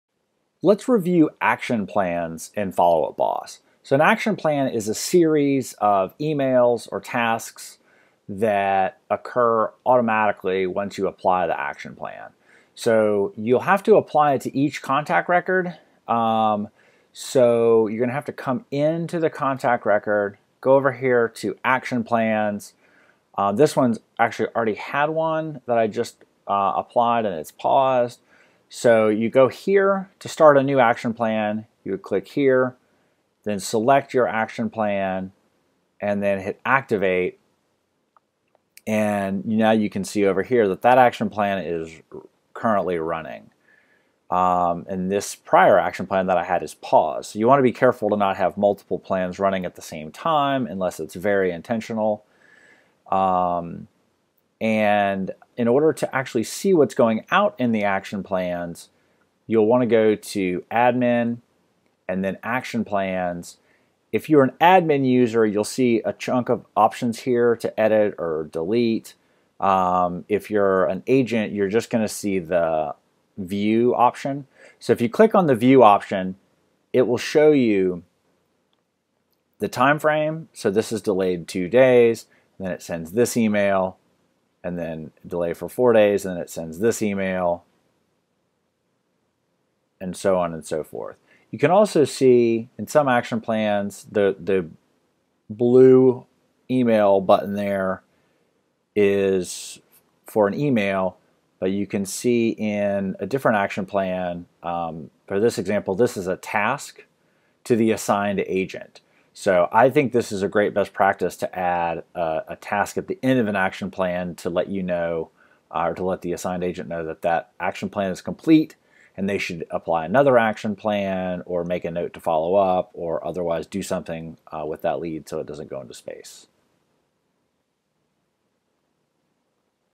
Follow Up Boss - Action Plans
This Video Will Review Action Plans in Follow Up Boss CRM and How to Use Them and View the Content Contained in Them.